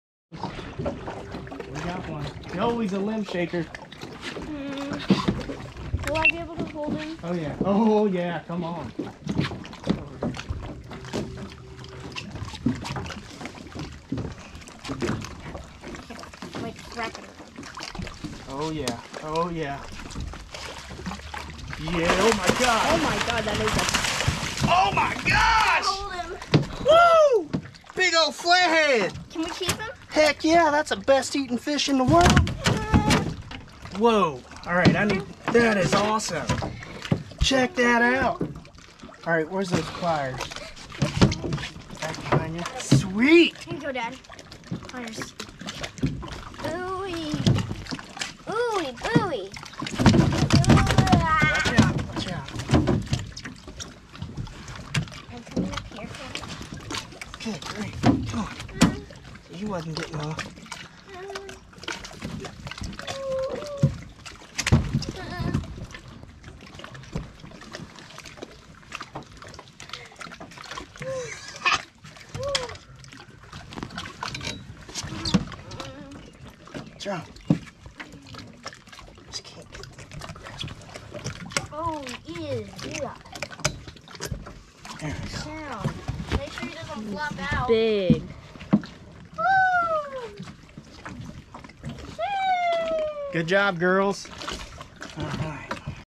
Limbline Flathead catfish, Lake of the Ozarks Warsaw Mo
Caught a nice flathead on limbline on shad.  Netted by 10 year old girl.
Fishing from a 1977 Landau Jon boat.
Used Mustad circle hooks.
Caught around the 80 mile mark of Lake of the Ozarks.
Time of Year: Mid-July in the 90's